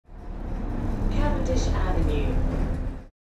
Cavendish Avenue
iBus announcement recorded on route K5 (SDE20283) on the 30/07/20